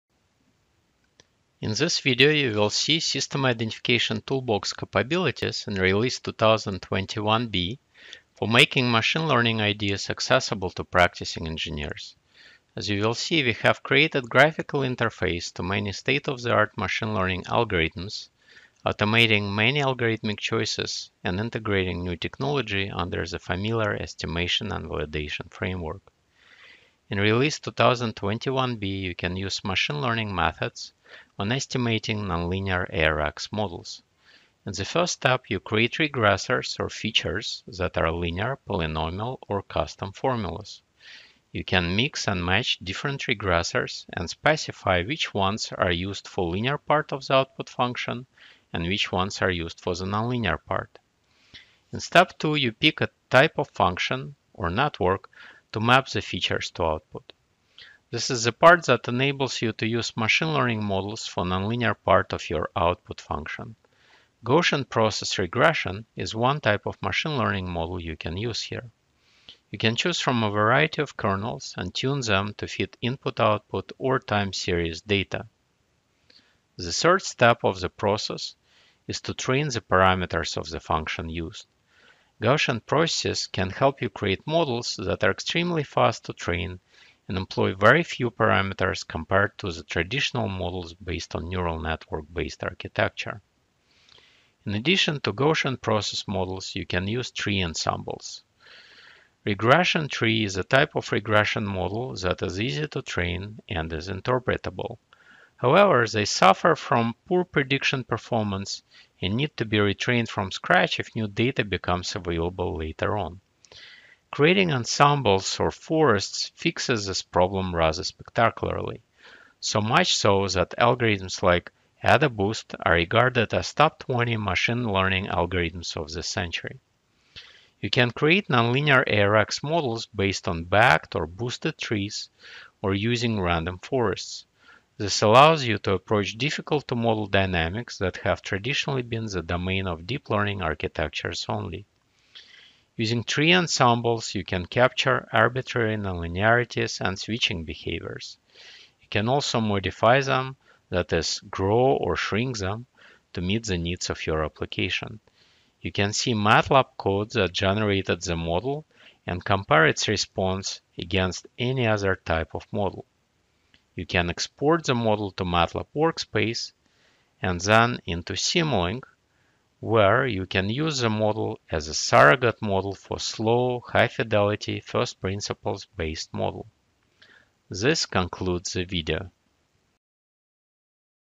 Machine Learning Techniques for System Identification
This video showcases NLARX updates and GP and ensemble methods,  machine learning techniques that are useful for system identification.